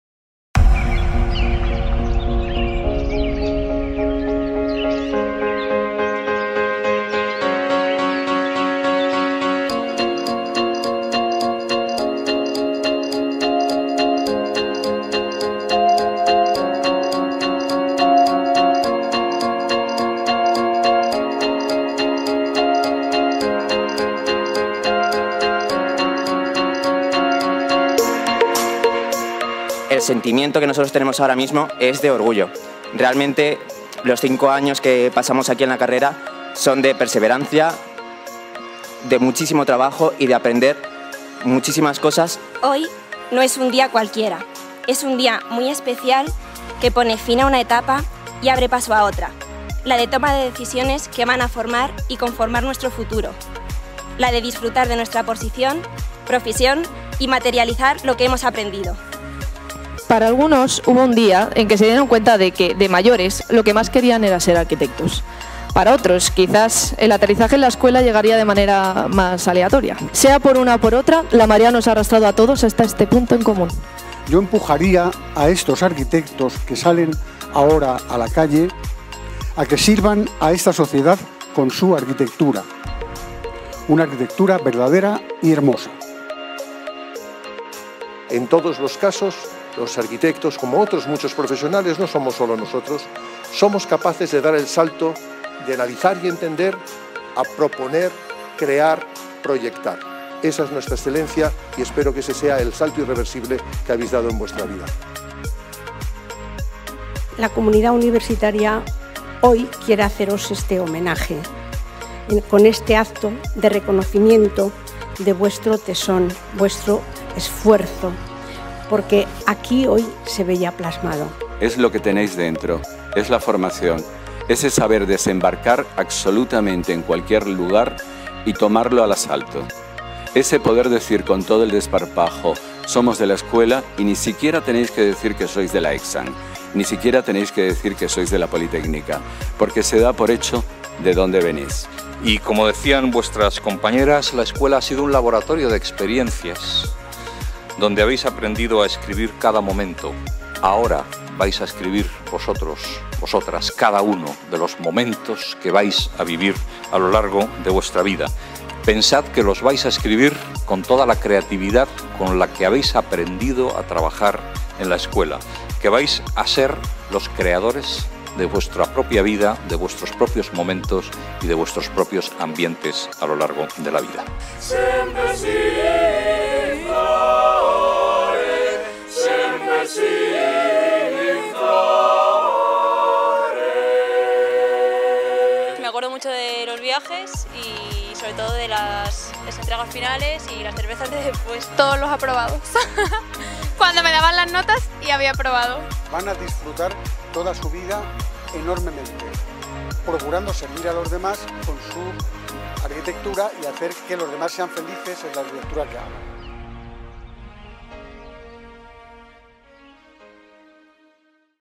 Acto Promoción Arquitectura UPM 2018 (aftermovie)
Vídeo resumen del Acto Homenaje de la CLXIX Promoción de la Escuela Técnica Superior de Arquitectura de la Universidad Politécnica de Madrid.

+info & follow